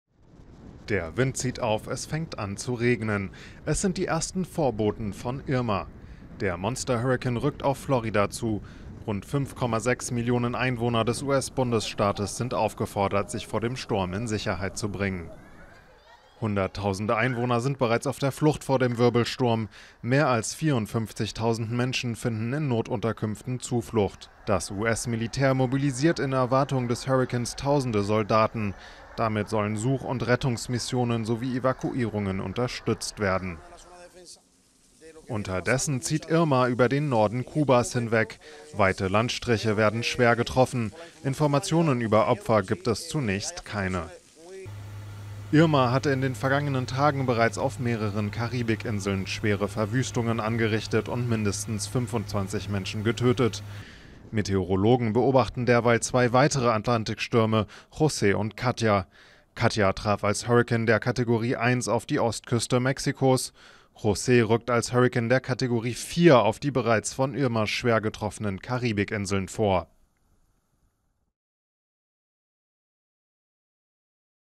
Florida in Angst vor "Irma"
In Florida wächst die Sorge vor Monster-Hurrikan "Irma": Rund 5,6 Millionen Einwohner des US-Bundesstaates wurden aufgefordert, sich vor dem Sturm in Sicherheit zu bringen. "Irma" war zuvor auf Kuba getroffen und richtete nach Behördenangaben schwere Schäden an, Opfer wurden zunächst nicht gemeldet.